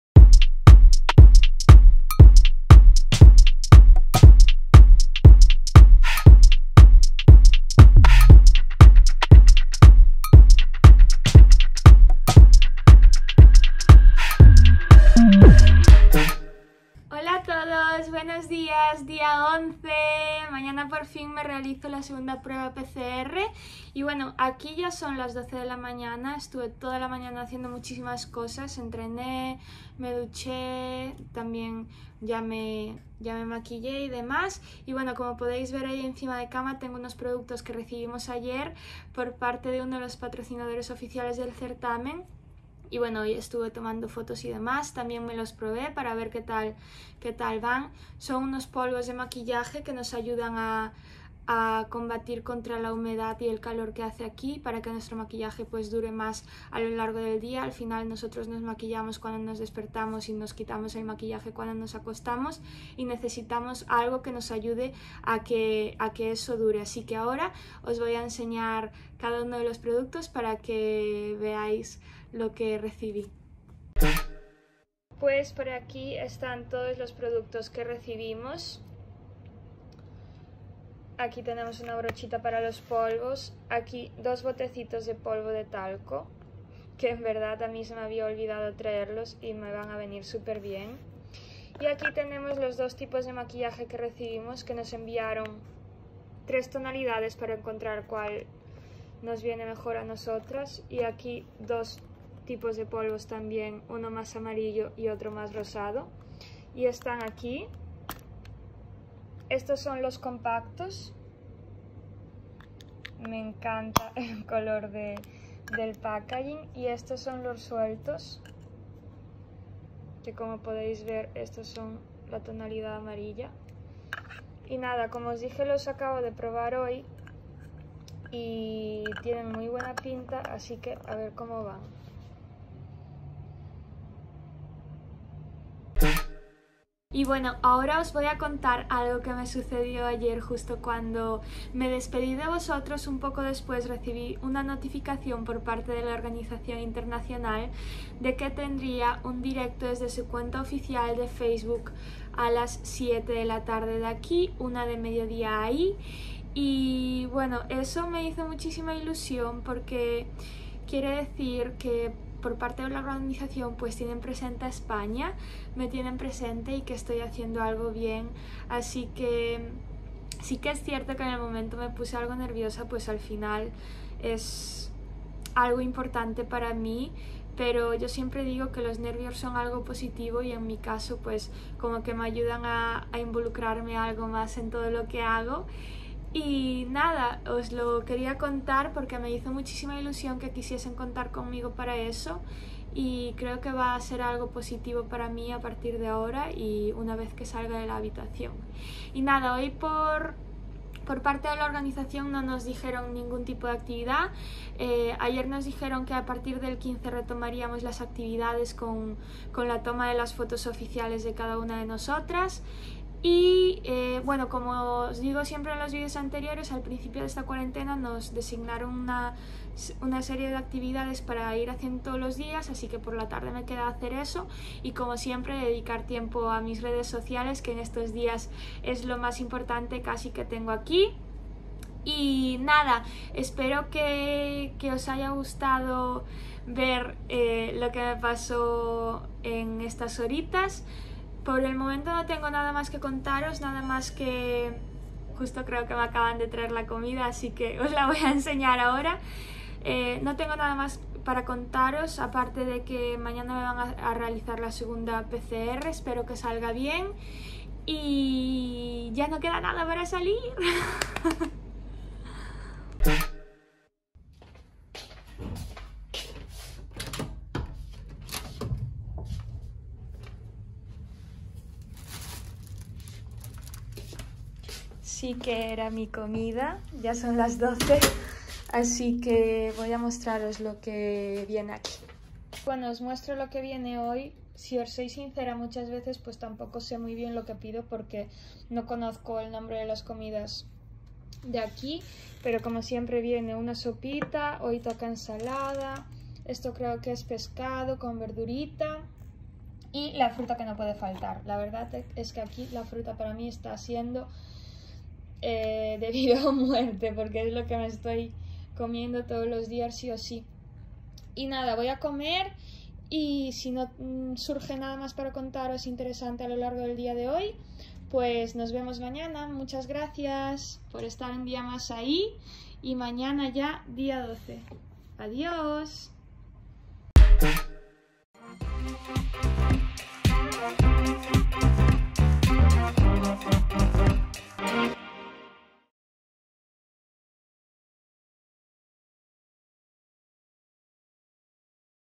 'Miss diarios´: El día a día de una modelo en Tailandia. Día 11
Iris Miguélez nos cuenta novedades y nos enseña los productos que le regaló la organización. Mañana se hará la segunda PCR y, muy pronto, las fotos oficiales del certamen. ¡Mirad qué guapa está Miss Grand Spain!